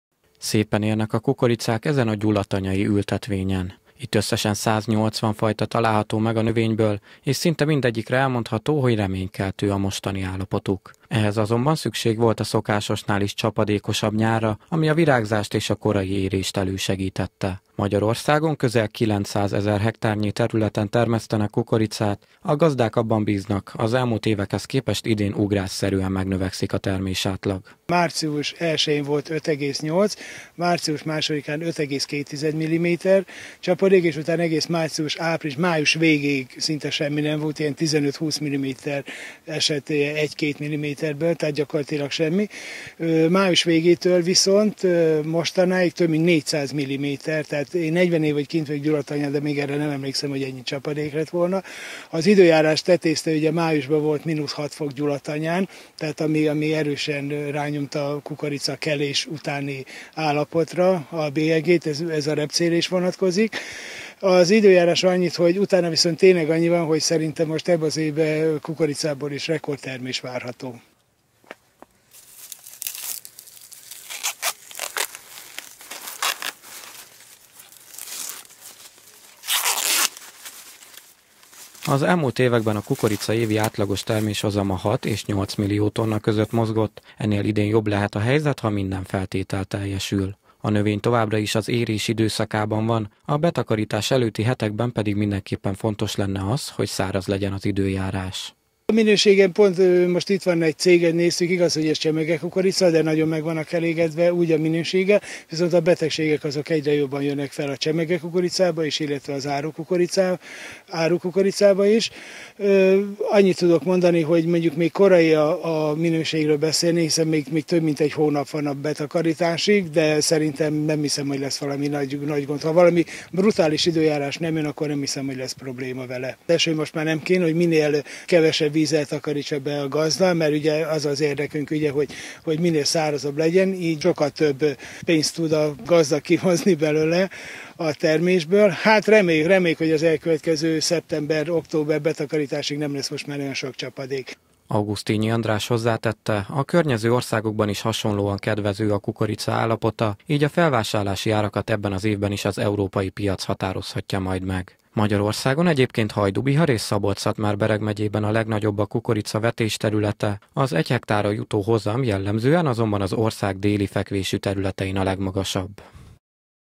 Szárazság kell a kukoricának - Kölcsey Televízió
Akár rekordmennyiségű kukoricát is betakaríthatnak idén a gazdák Magyarországon. Ehhez azonban az kell, hogy a következő hetekben szárazabb legyen az időjárás. A növény csőképzésének jót tett a nyár első felében lehullott sok csapadék, az elmúlt napokban azonban már inkább károkat okozott az eső, vallják a gazdák.